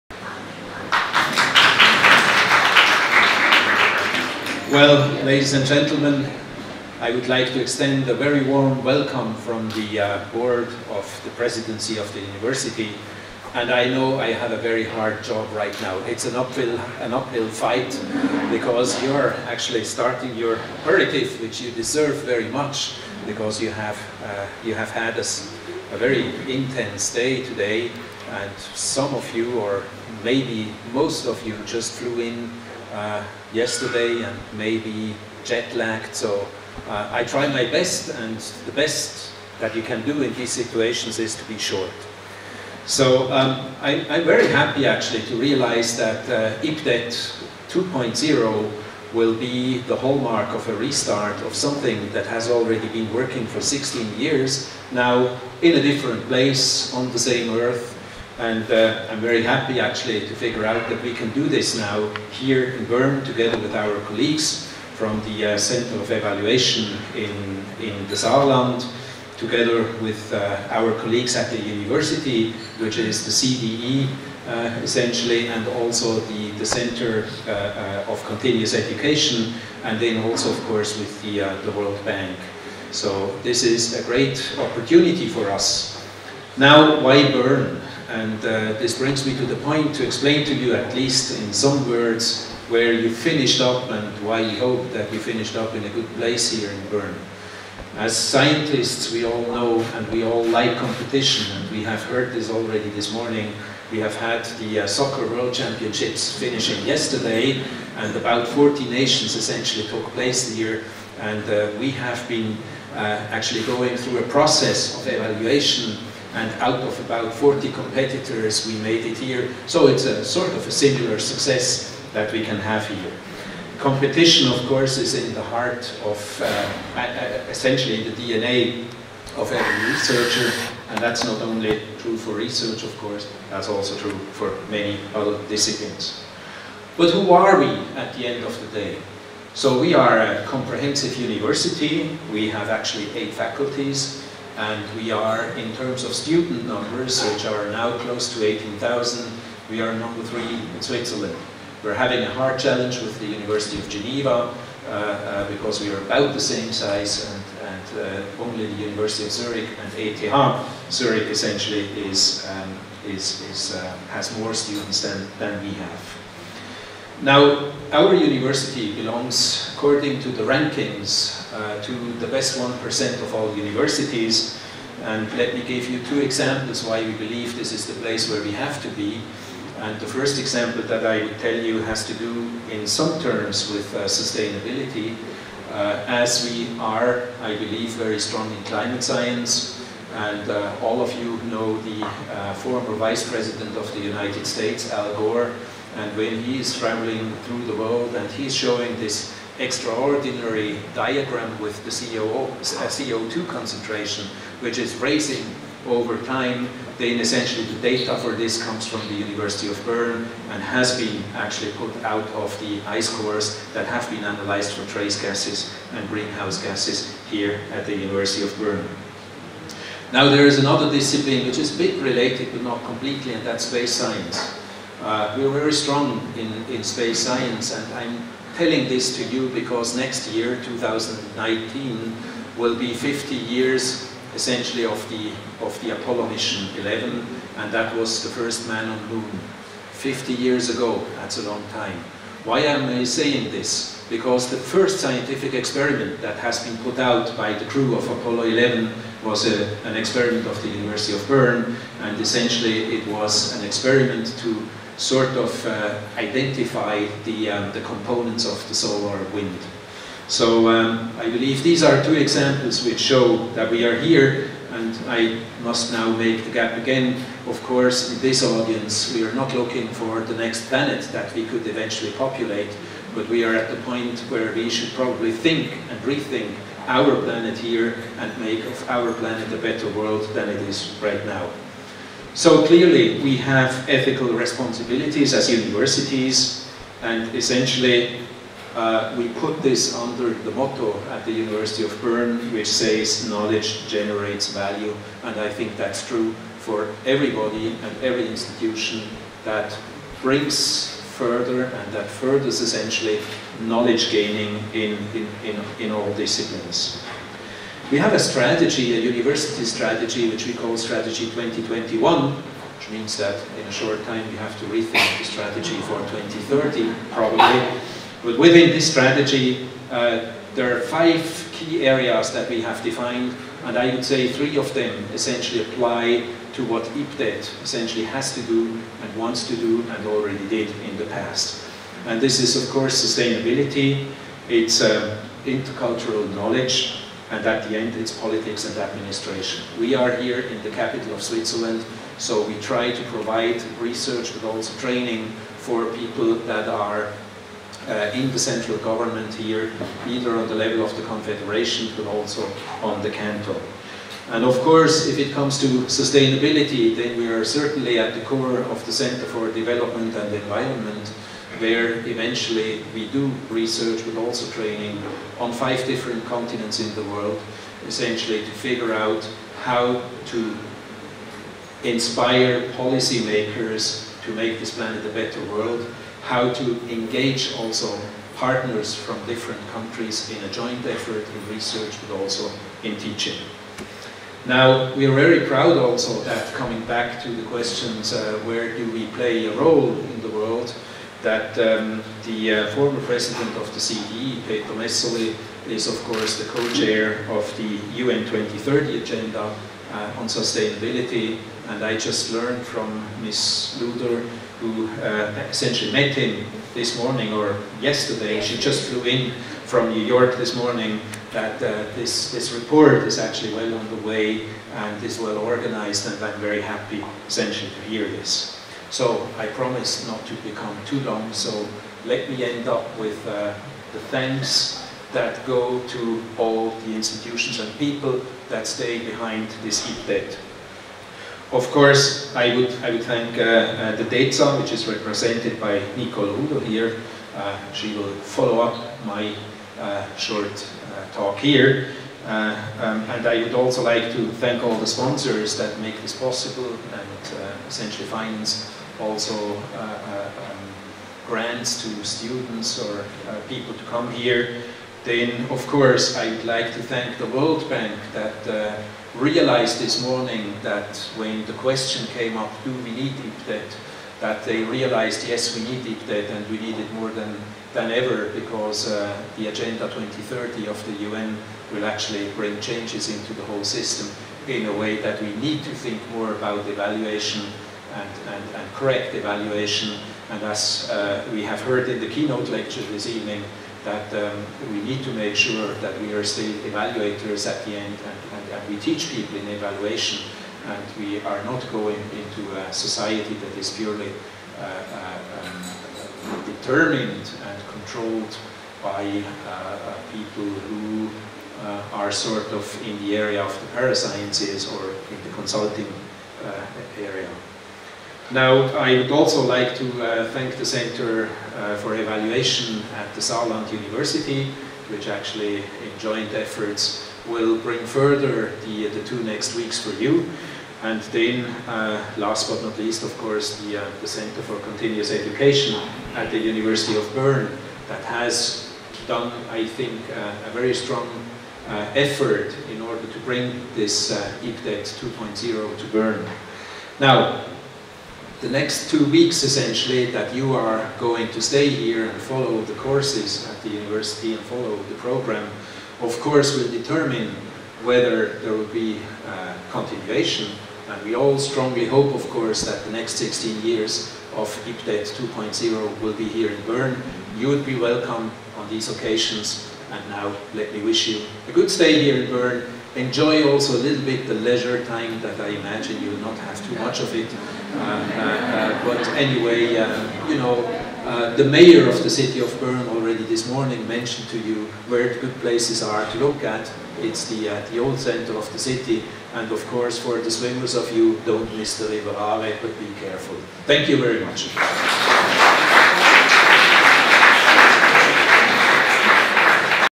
IPDET 2018: Welcome address by Prof. Dr. Christian Leumann
Welcome address by Prof. Dr. Christian Leumann, Rector of the University of Bern